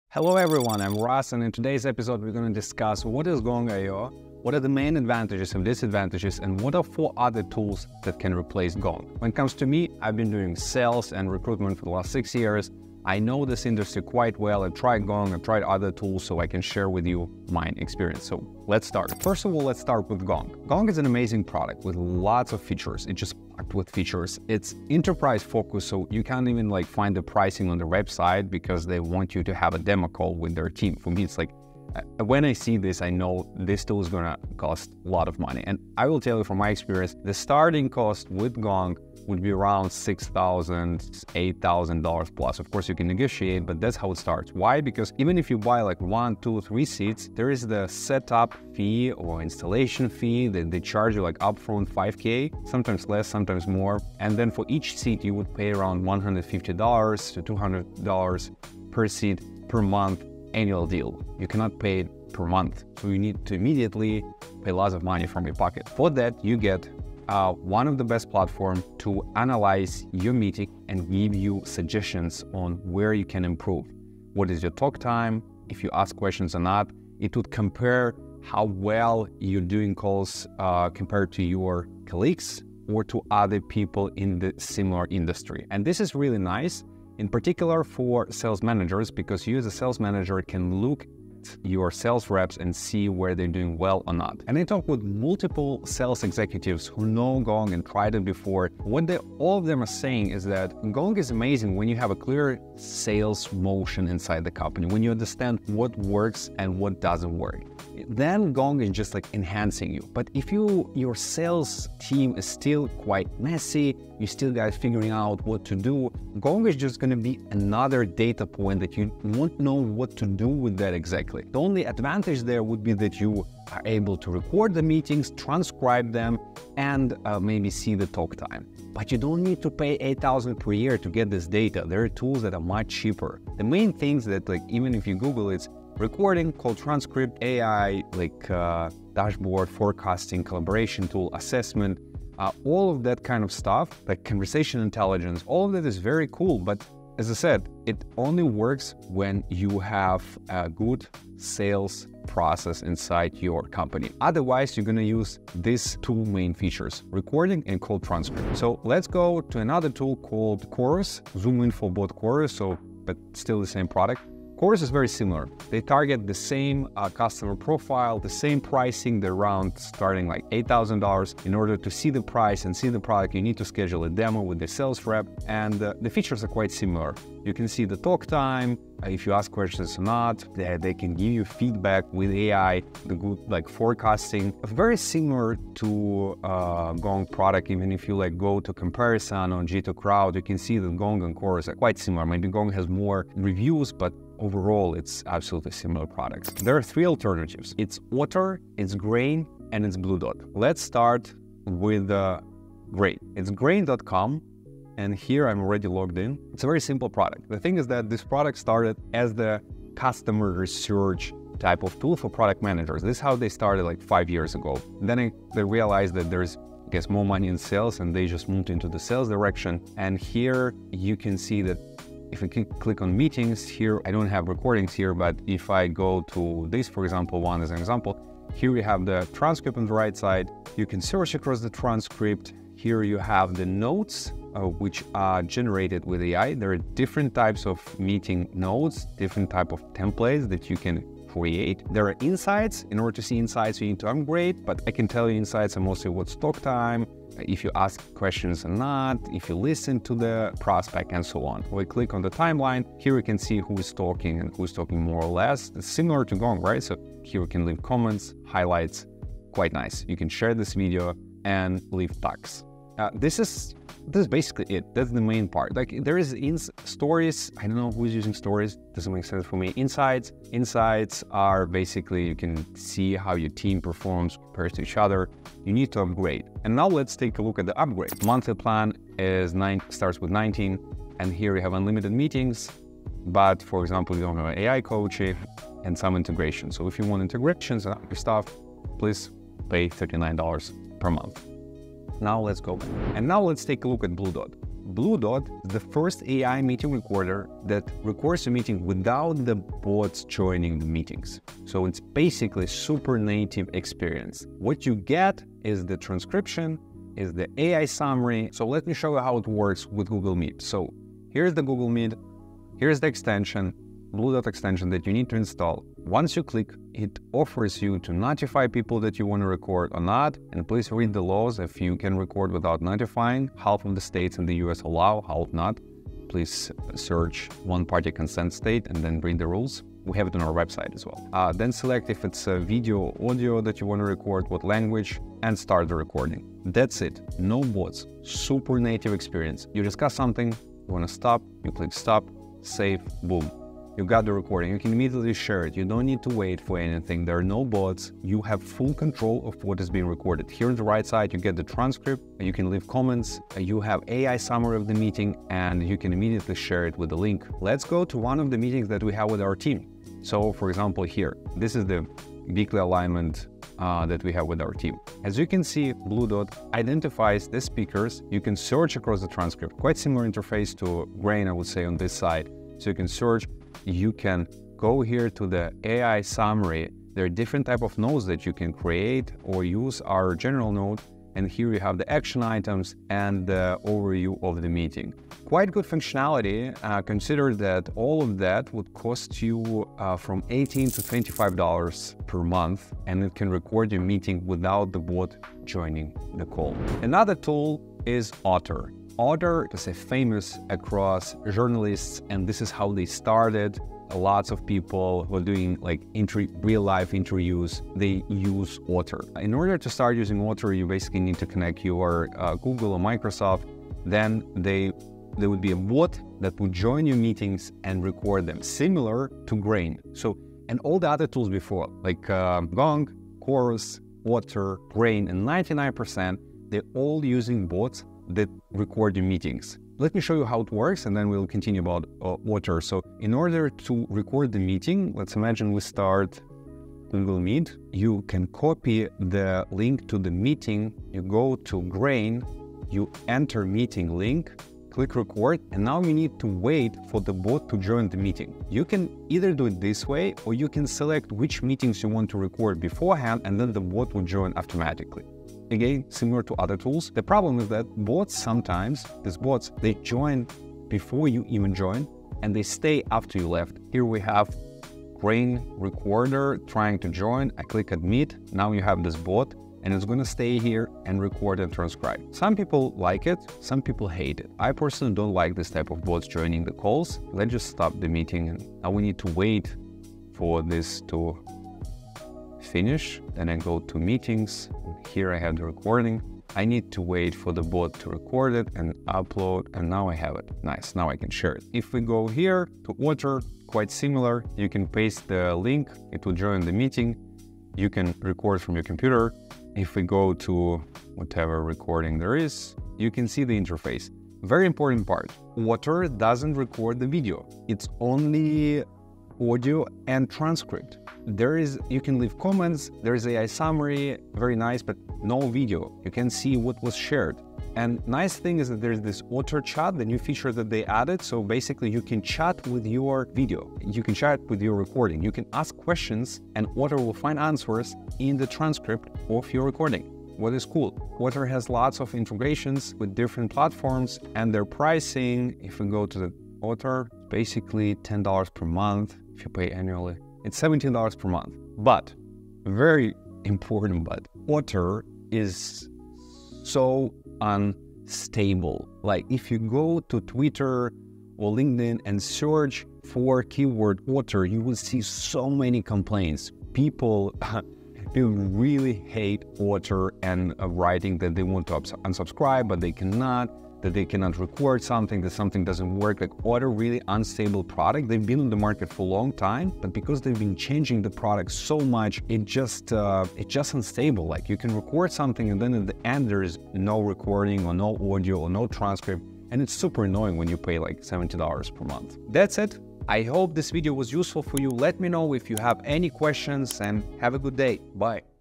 Top 4 Gong Alternatives in 2024: Chorus vs Otter vs Grain vs Bluedot
Looking for the best Gong alternatives? Check out this comparison of Chorus, Otter, Grain, and Bluedot for conversation intelligence in 2024!

We will discuss pricing, quality of transcription, user experience, and all important features.

0:00 - Intro
0:18 - Gong Intro
0:30 - Gong Pricing
1:30 - Who Should Use Gong
2:15 - Gong Main Features
2:40 - Chorus Review
3:15 - Grain Review
4:54 - Bluedot Review
6:37 - Otter Review
7:00 - How Bots Record The meetings

Here is the original article with Top 8 Top Gong Alternatives